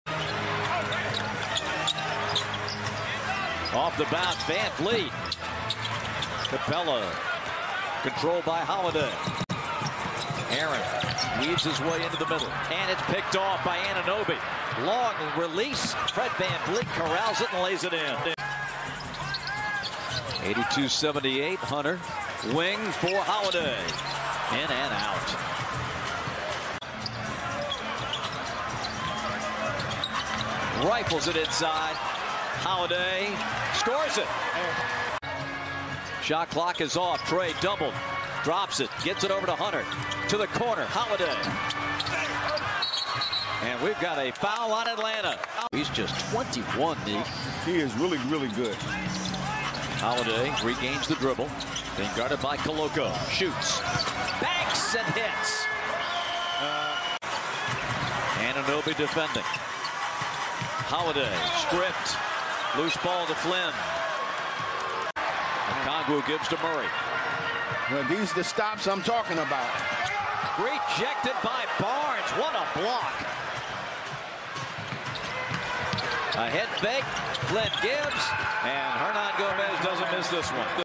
Aaron Holiday : All Possessions (2022-11-19)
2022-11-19 Atlanta Hawks vs Toronto Raptors: Player Highlights Aaron Holiday
    0 PTS, 1 REB, 0 AST, 0 STL.
        0 of 0 from 3.
        0 of 0 FGs in 8:00 minutes.